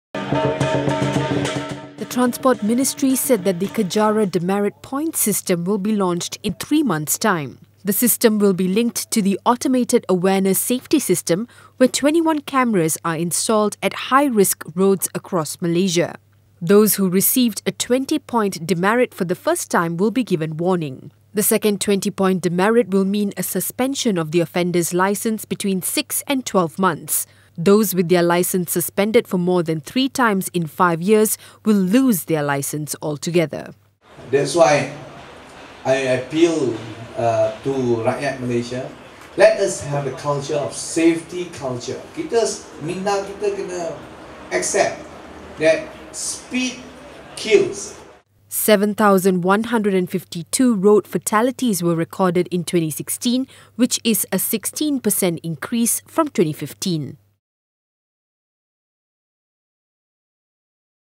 Demerit points system coming in three months
The Transport Ministry said that traffic offenders will be punished under the Kejara demerit points system that will be launched in three months’ time.